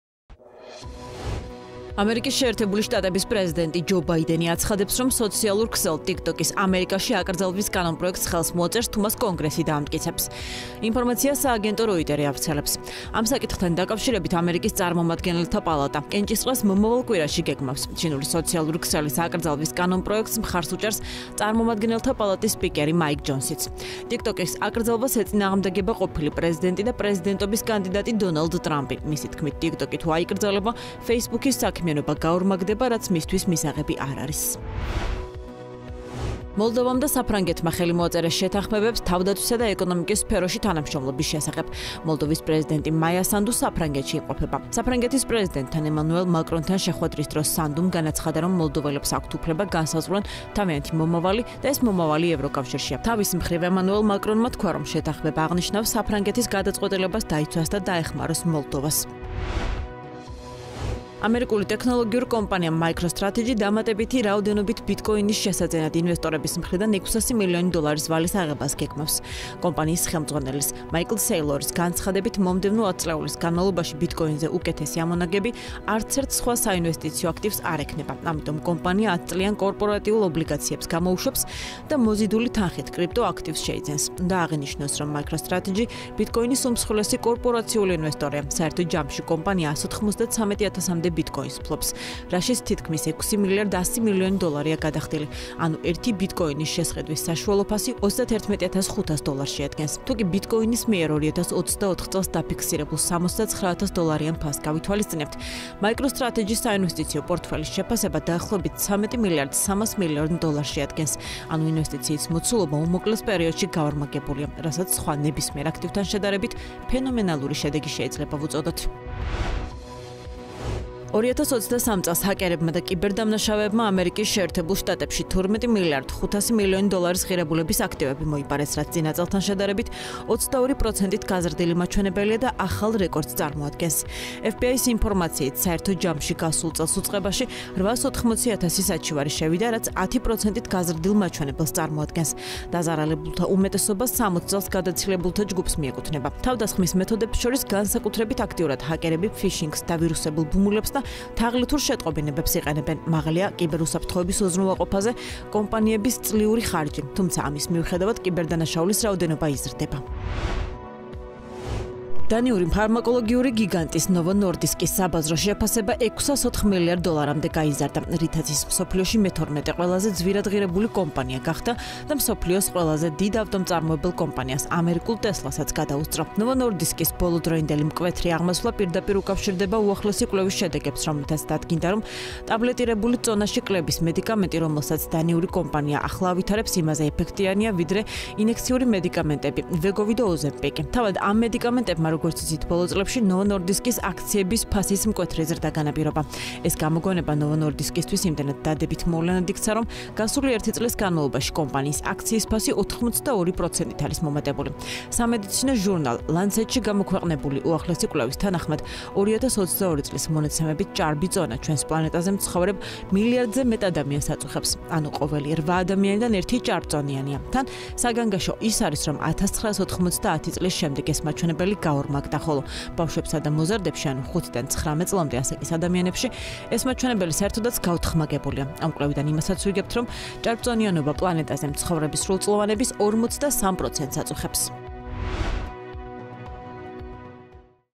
საერთაშორისოამბები
🔹ბაიდენი აცხადებს, რომ აშშ-ში „ტიკ-ტოკის“ აკრძალვის კანონპროექტს ხელს მოაწერს;
🔹მოლდოვამ და საფრანგეთმა თანამშრომლობის შეთანხმებებს ხელი მოაწერეს;
🔹MicroStrategy ბიტკოინის შესაძენად $600 მილიონის ვალის აღებას გეგმავს;
🔹2023 წელს ჰაკერებმა აშშ-ში $12.5 მილიარდი მოიპარეს;
🔹Novo Nordisk-ის ღირებულება რეკორდულად გაიზარდა, მიზეზი - წონაში კლების წამალია;